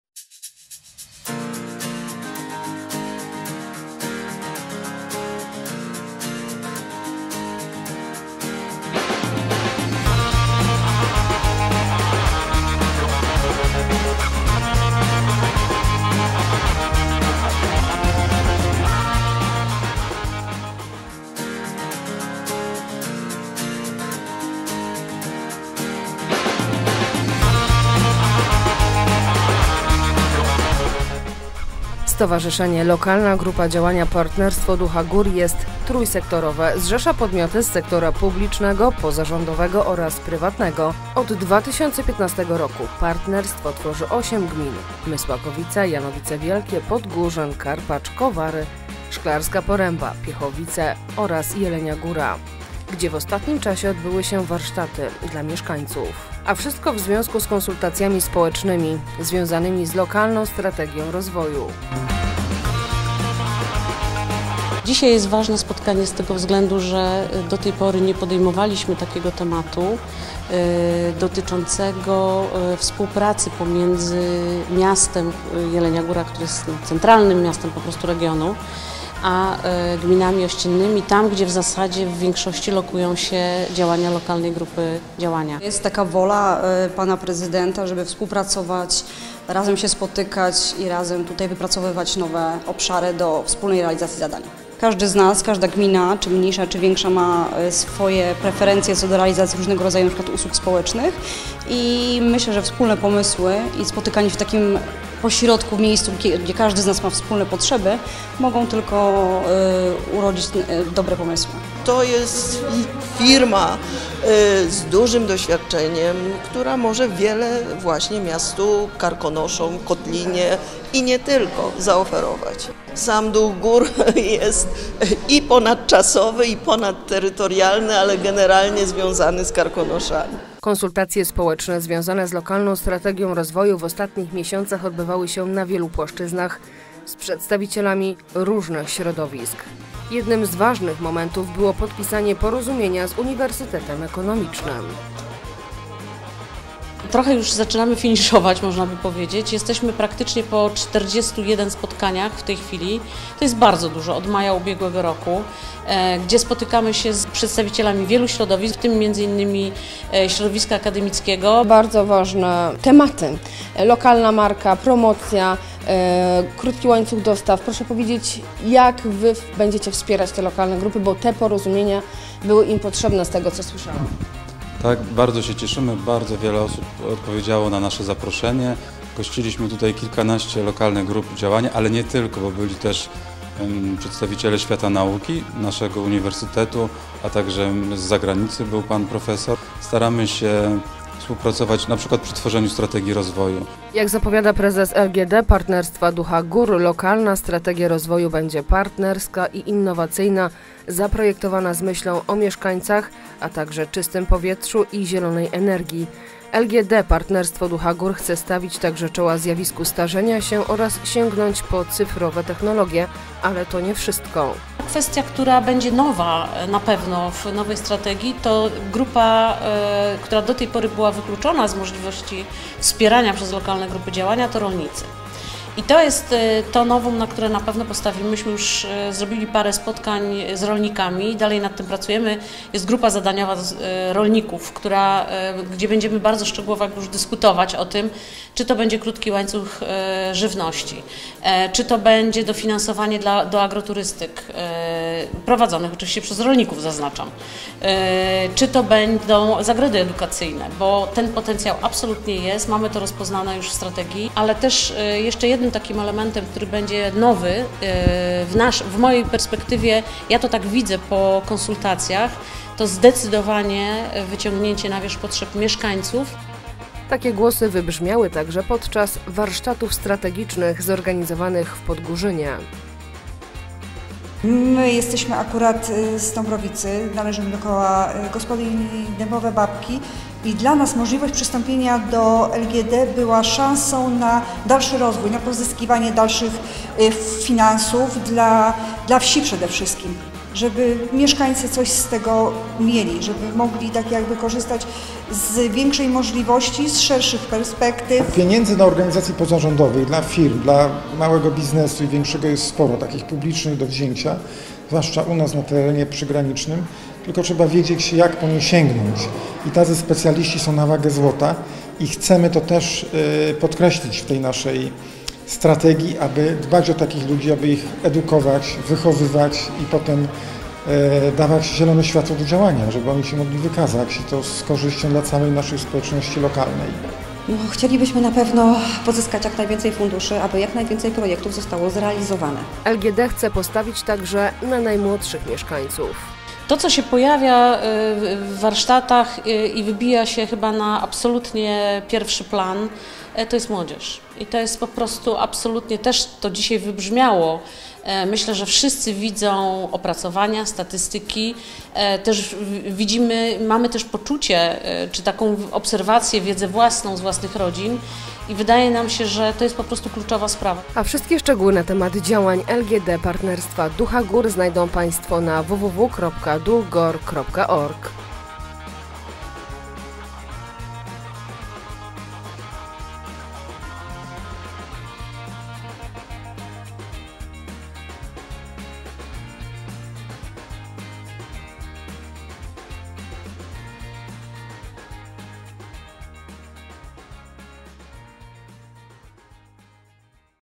Lokalna Strategia na lata 2023-2027
Stowarzyszenie Lokalna Grupa Działania Partnerstwo Ducha Gór to partnerstwo trójsektorowe: zrzeszające osoby/podmioty z sektora publicznego, pozarządowego oraz prywatnego. Od 2015 r. partnerstwo tworzy 8 gmin/miast: Mysłakowice, Janowice Wielkie, Podgórzyn, Karpacz, Kowary, Szklarska Poręba, Piechowice, Jelenia Góra, gdzie w ostatnim czasie odbyły się warsztaty dla mieszkańców.